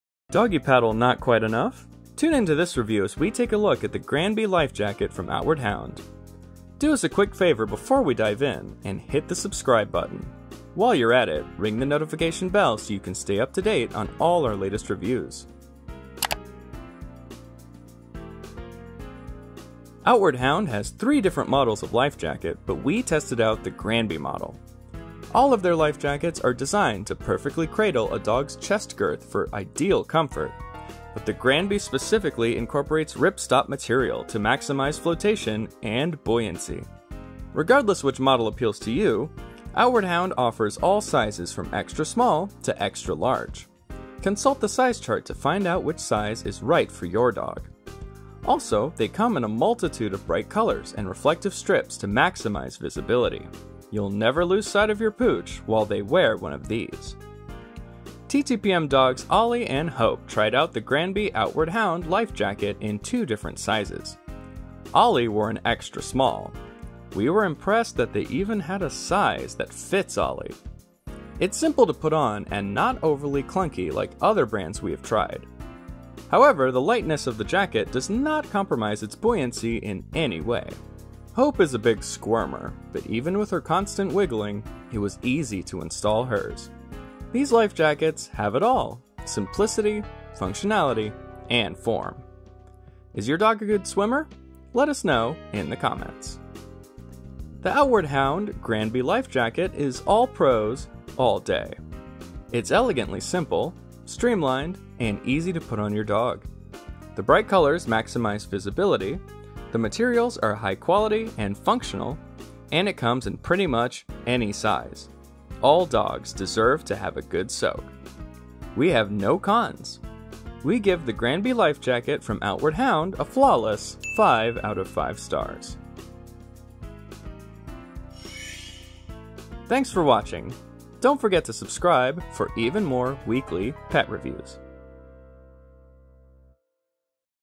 Granby Dog Life Jacket from Outward Hound Review!
Doggie paddle not quite enough? Tune into this review as we take a look at the Granby Life Jacket from Outward Hound. For full review and shopping info

Product Info: Outward Hound's Granby Life Jacket is lightweight, streamlined, and incorporates ripstop material to maximize flotation and buoyancy. It comes in all sizes, from XS to XL.